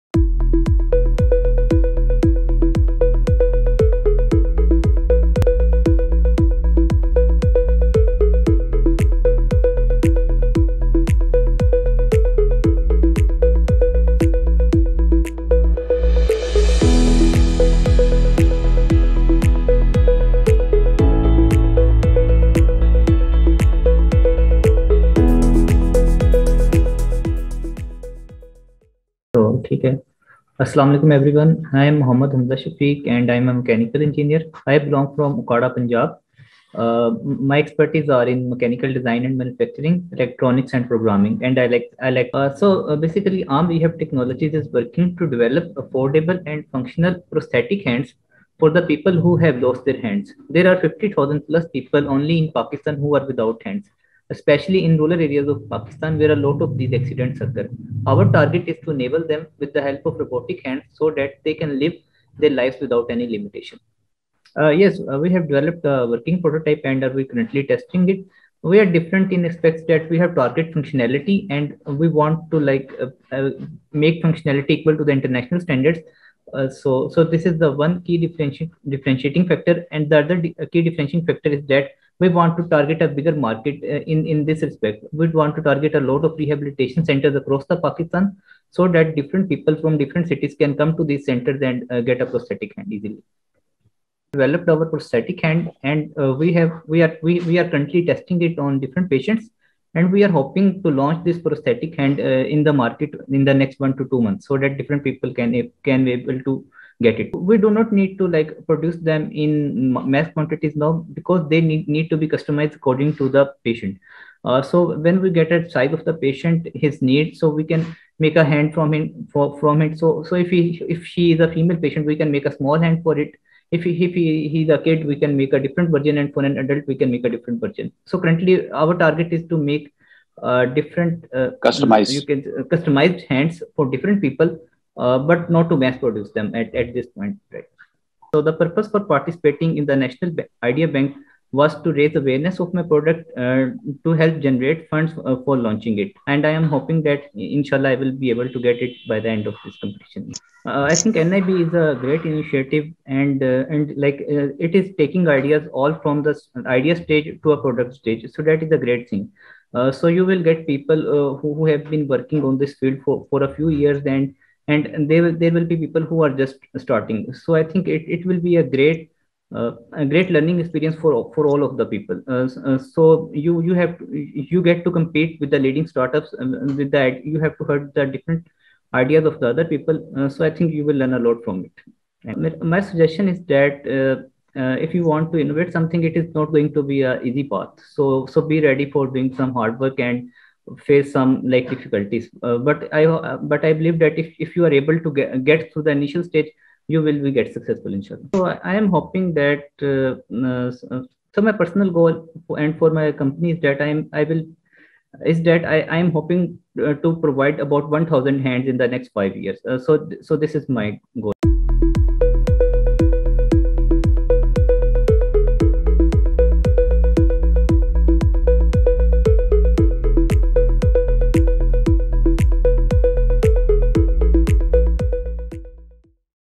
Muhammad Hamza Shafique
Muhammad Hamza Shafique is the 2nd national winner from Sector Human Health. He is the CEO at Arm Rehab Technology.